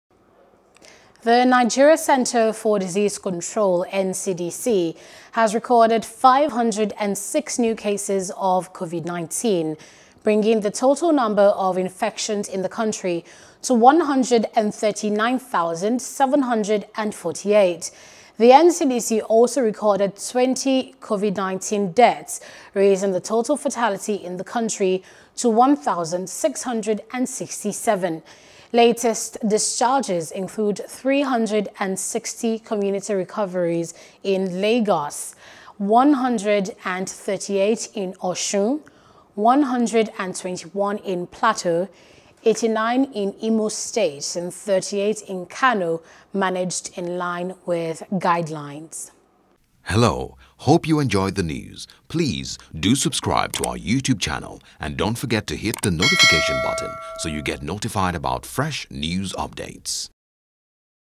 Covid – 19 Update: Nigeria Records 506 New Covid - 19 Cases
The Nigeria Centre for Disease Control (NCDC) has recorded 643 new cases of coronavirus COVID-19, bringing the total number of infected people in the country to 140,391.